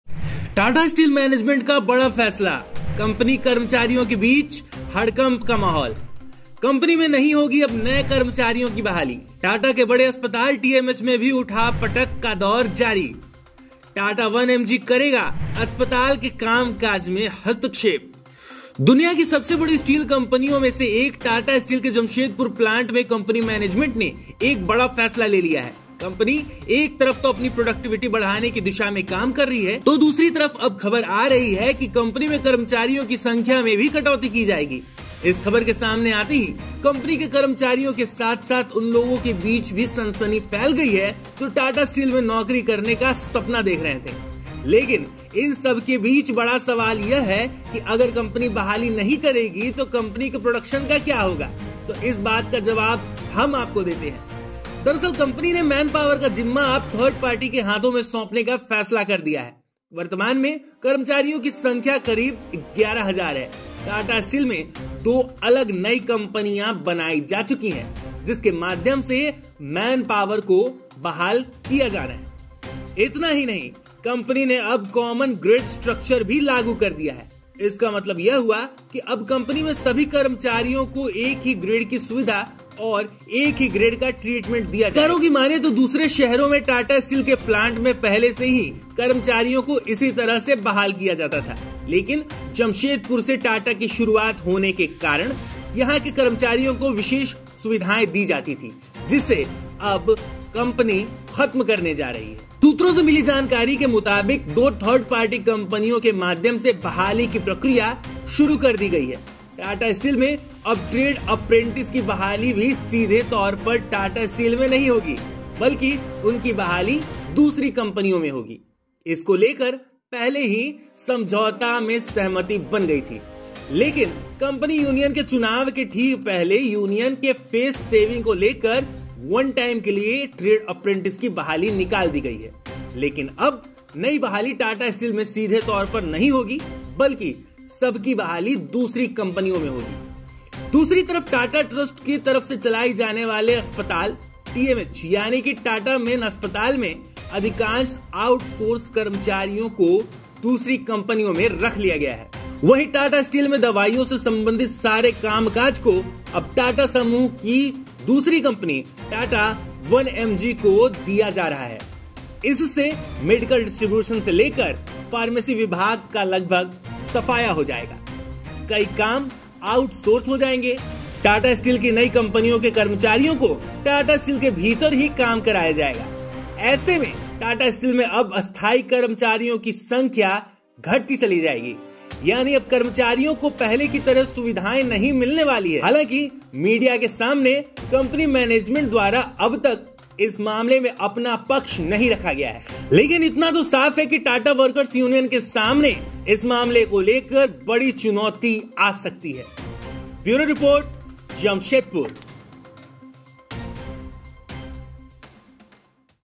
टाटा स्टील मैनेजमेंट ने एक साथ लिए कई बड़े फैसले || Tata Steel News || Jamshedpur Jharkhand News
Tata Steel ने एक साथ लिए कई बड़े फैसले, जमशेदपुर के लोगों पर होगा इसका सीधा असर। पूरी खबर जानने के लिए देखिए इस रिपोर्ट को

Tata Steel News || Jamshedpur Jharkhand News

टाटा स्टील मैनेजमेंट ने एक साथ लिए कई बड़े फैसले